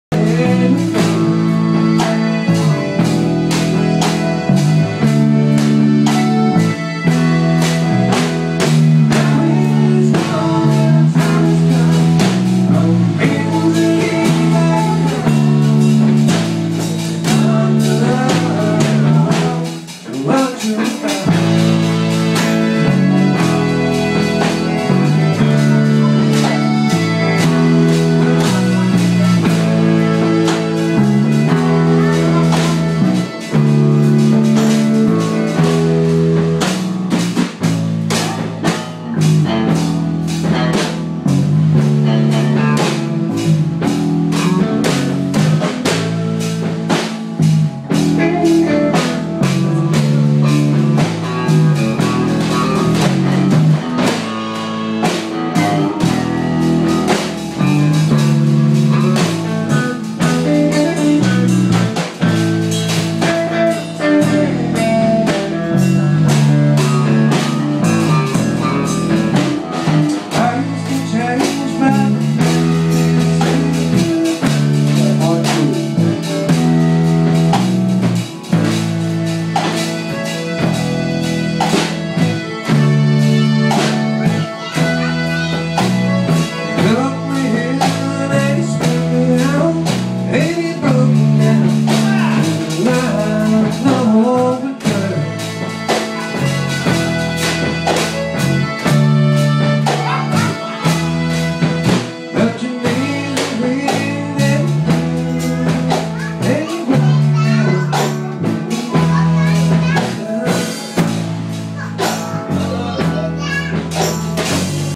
Atticus and Evie
Atticus drums, Evie dances